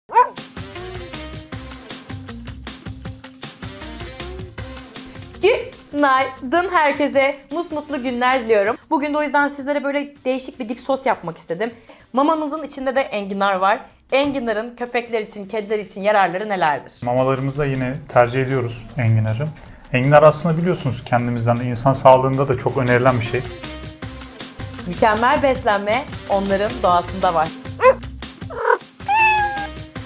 Patili Tarifler: Enginarlı Dip Sos
Malzemeler;
- 300 gr ıspanak
- 2 - 3 adet enginar
- Yarım paket sürme beyaz peynir
- Kaşar peyniri (İstediğiniz kadar)
- Parmesan (3 yemek kaşığı kadar damak zevkinize göre azaltıp ekleyebilirsiniz.)
- 1 paket krema
- Baharatlar (Kendi damak zevkinize uygun bir şekilde 1 tatlı kaşığı kadar ekleyebilirsiniz.)
- Tuz
- Karabiber
- Soğan tozu
- Sarımsak tozu

Hazırlanışı;
Ispanak ve enginarı minik bir şekilde doğrayın. Doğradığınız parçaları az yağda öldürerek kenarı alın. Karışımın içerisine dilediğiniz peyniri ilave edin. Krema ve baharatları ekleyin. Tüm karışımı fırın kabına alın. Üzerine ekstra kaşar peyniri ekleyerek fırında peynirler eriyene kadar pişirin. Afiyet olsun!